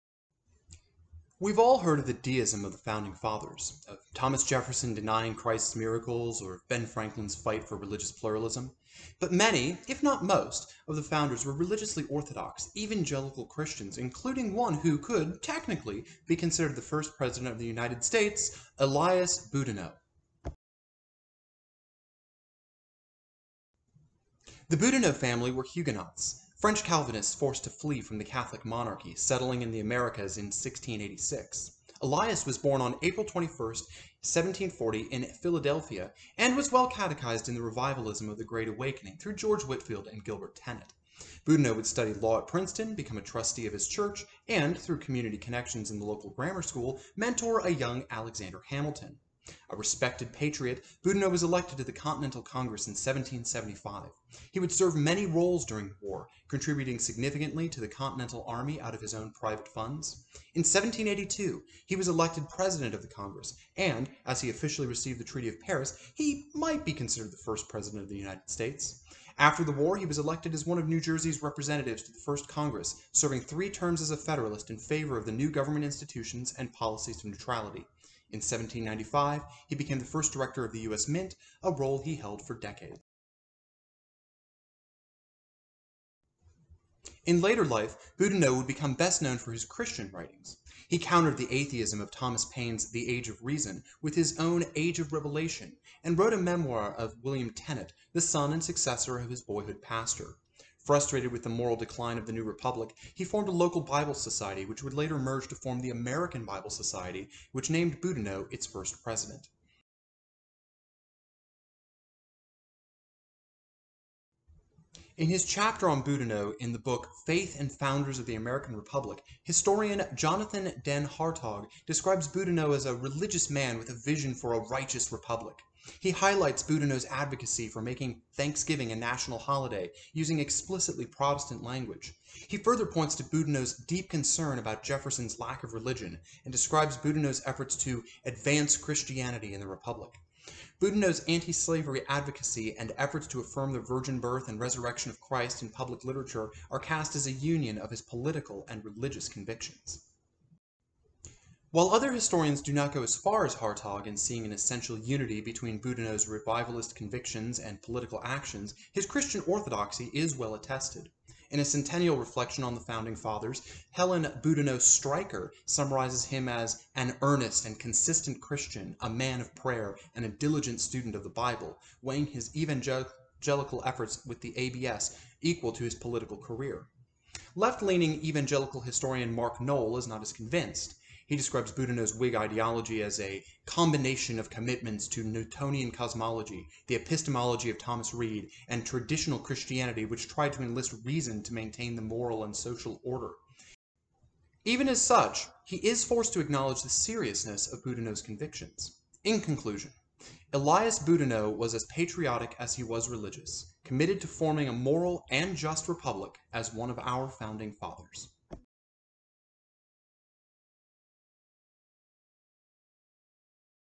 Elias Boudinot, Evangelical Founder
Not all the founders were deists - many were evangelical Christians.  Learn about Elias Boudinot, early Continental Congress president and founder of the American Bible Society

Submission in partial fulfillment of requirements for HIUS 512: American Revolution at Liberty University

References:
Jonathan Den Hartog, "Elias Boudinot, Presbyterians, and the Quest for 'Righteous Republic,'" in Faith and the Founders of the American Republic eds. Mark D. Hall and Daniel L. Dreisbach (New York: Oxford University Press, 2014): 253-276

Mark A. Noll, "The Response of Elias Boudinot to the Student Rebellion of 1807: Visions of Honor, Order, and Morality," The Princeton University Library Chronicle 43 no. 1 (1981): 15

Helen B. Stryker, "Elias Boudinot," The Pennsylvania Magazine of History and Biography 3, no. 2 (1879): 193